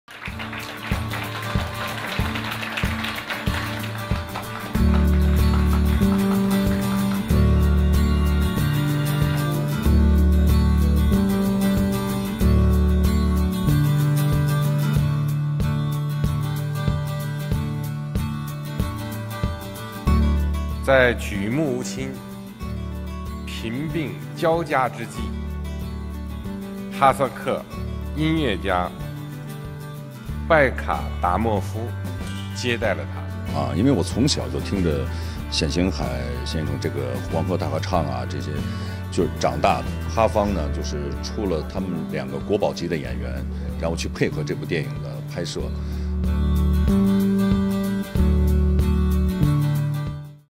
'Composer': First Chinese-Kazakh co-production starts filming in Astana
The first co-production between China and Kazakhstan has started filming in the capital Astana.

"Composer" is a biographical film which tells the the little-known story of the later years of Chinese musician Xian Xinghai, covering the period he spent in Kazakhstan and Russia between 1940 and 1945.

Beijing-based Shinework Pictures has teamed up with Kazakhfilm JSC, the largest film production studio in Kazakhstan, to co-produce the movie.

The movie is partly inspired by a speech Chinese President Xi Jinping gave at Astana's Nazarbayev University in 2013, where he spoke of Xian's links with the country.

The Chinese and Kazakh governments are promoting more cultural exchanges and people-to-people communication.

The movie is set to premiere on June 13, 2018.